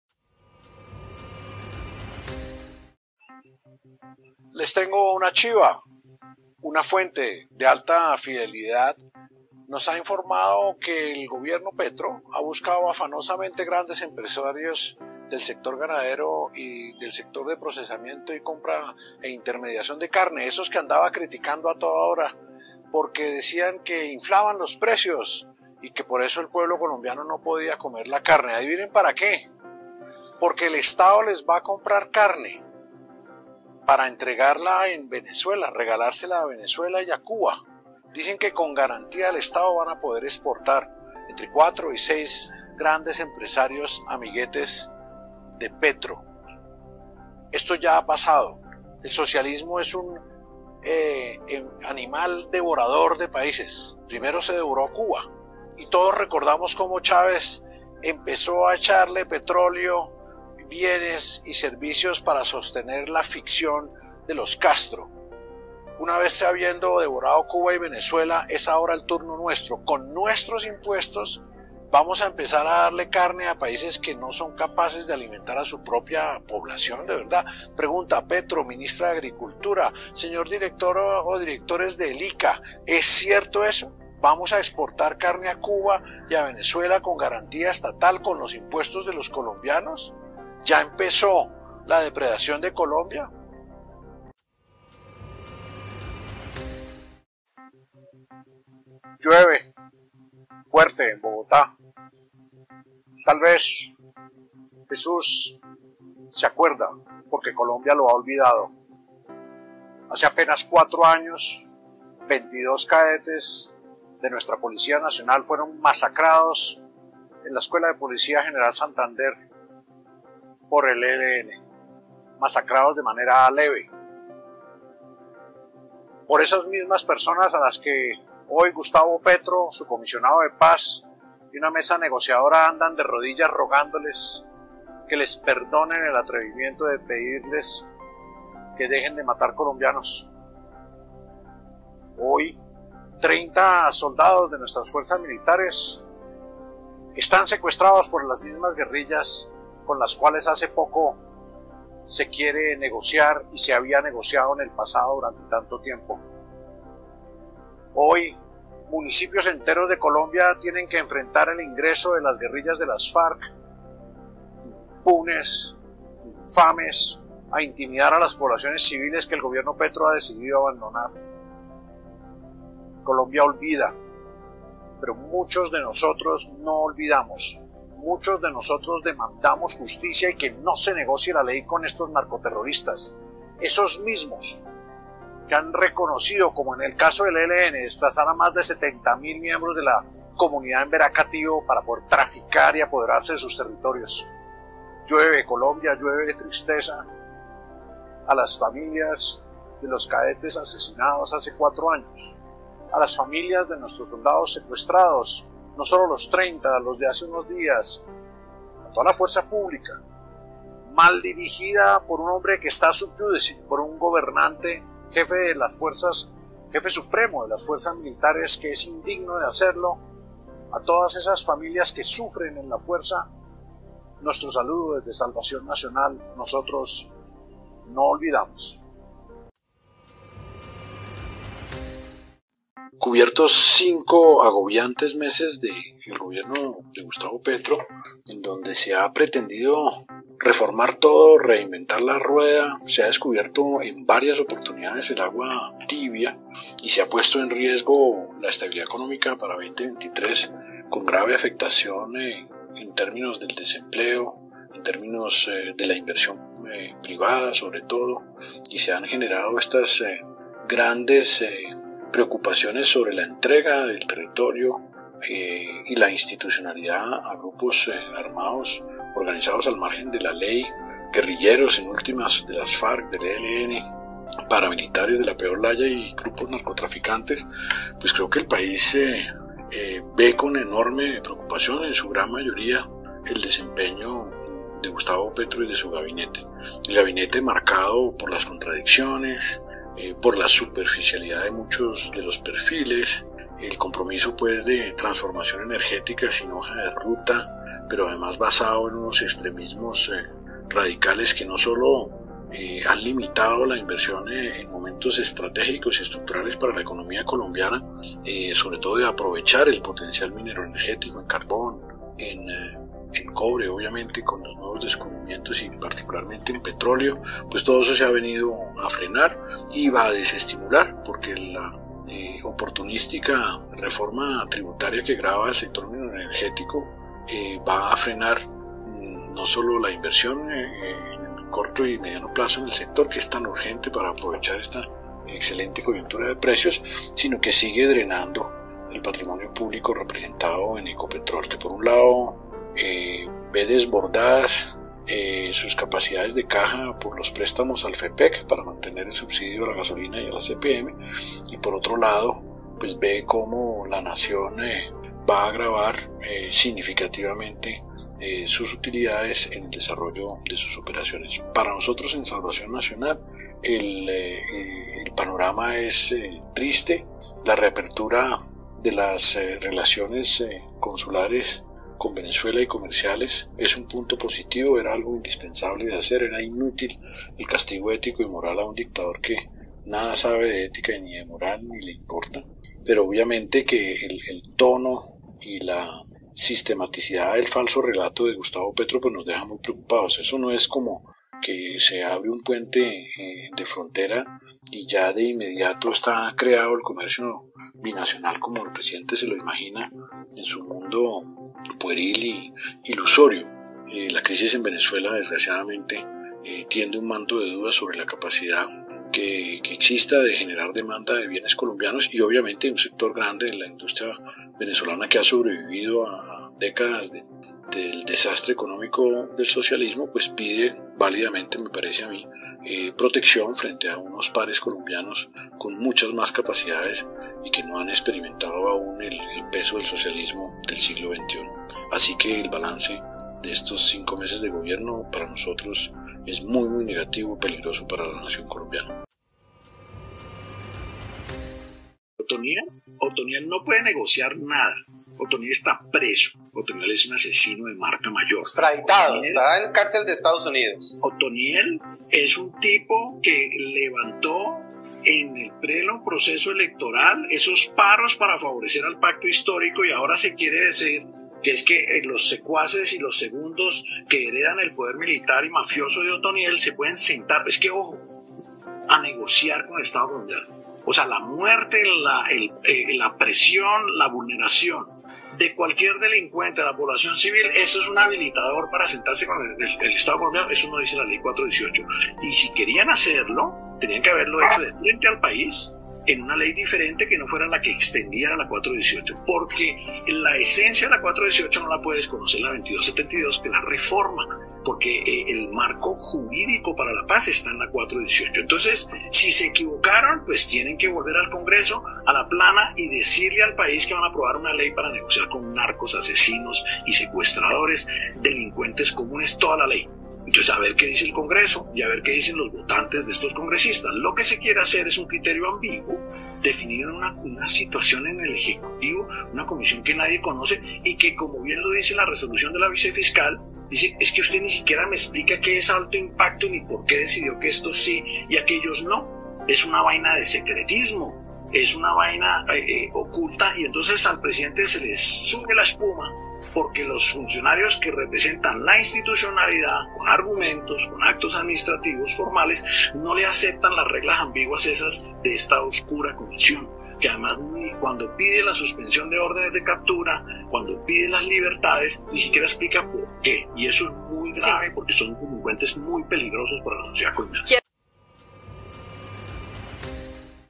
🔴 4 FUERTES DENUNCIAS DE ENRIQUE GÓMEZ
El comienzo de este desgobierno está atravesado por los lugares comunes anunciados con bombos y platillos, algo que es hasta común, pero lo delicado es la entrega de la institucionalidad, el arrodillamiento de la Fuerza Pública lo que conlleva por supuesto al más delito.